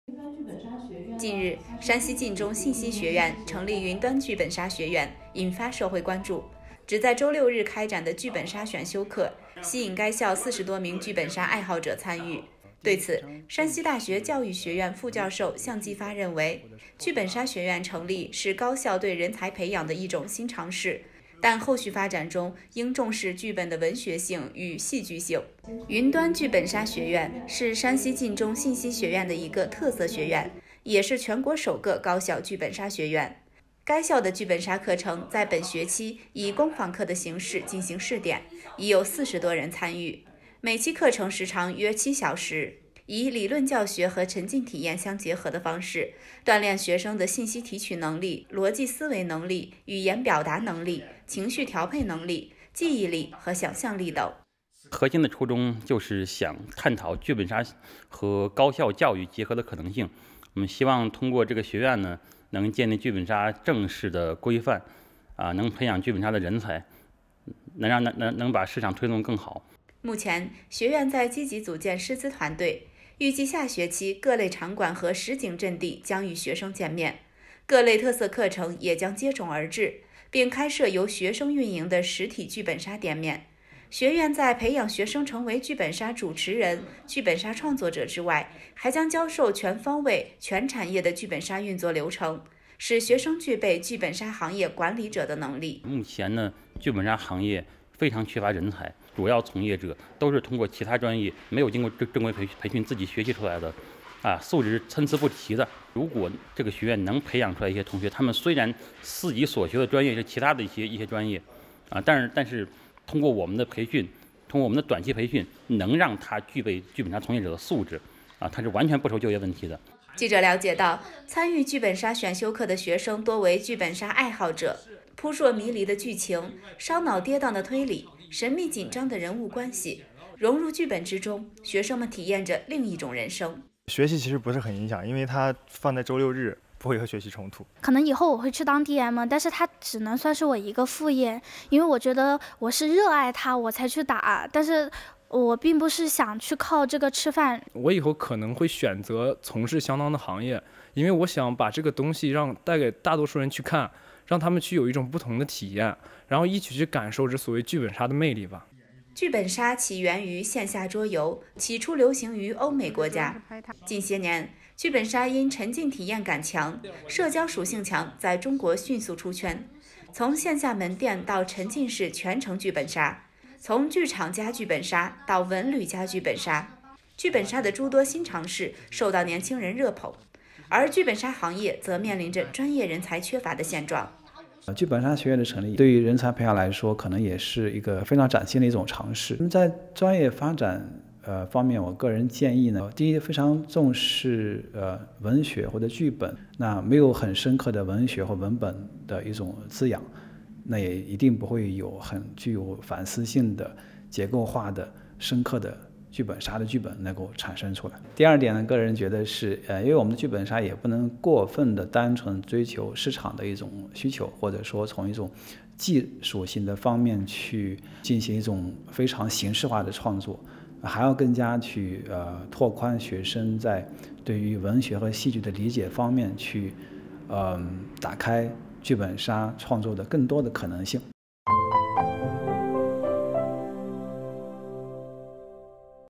山西一高校成立剧本杀学院 来听专家怎么说！
近日，山西晋中信息学院成立云端剧本杀学院，引发社会关注。只在周六日开展的剧本杀选修课，吸引该校40多名剧本杀爱好者参与。对此，山西大学教育学院副教授项继发认为，剧本杀学院成立是高校对人才培养的一种新尝试，但后续发展中应重视剧本的文学性与戏剧性。

云端剧本杀学院是山西晋中信息学院的一个特色学院，也是全国首个高校剧本杀学院。该校的剧本杀课程在本学期以工坊课的形式进行试点，已有40多人参与。每期课程时长约7小时，以理论教学和沉浸体验相结合的方式，锻炼学生的信息提取能力、逻辑思维能力、语言表达能力、情绪调配能力、记忆力和想象力等。

目前，学院在积极组建师资团队。预计下学期各类场馆和实景阵地将与学生见面，各类特色课程也将接踵而至，并开设由学生运营的实体剧本杀店面。学院在培养学生成为剧本杀主持人、剧本杀创作者之外，还将教授全方位、全产业的剧本杀运作流程，使学生具备剧本杀行业管理者的能力。

记者了解到，参与剧本杀选修课的学生多为剧本杀爱好者。扑朔迷离的剧情、烧脑跌宕的推理、神秘紧张的人物关系……融入剧本之中，学生们体验着另一种人生。

剧本杀起源于线下桌游，起初流行于欧美国家。近些年，剧本杀因沉浸体验感强、社交属性强在中国迅速“出圈”。从线下门店到沉浸式全城剧本杀，从“剧场+剧本杀”到“文旅+剧本杀”，剧本杀的诸多新尝试受到年轻人热捧，而剧本杀行业则面临着专业人才缺乏的现状。（王惠琳 杨静）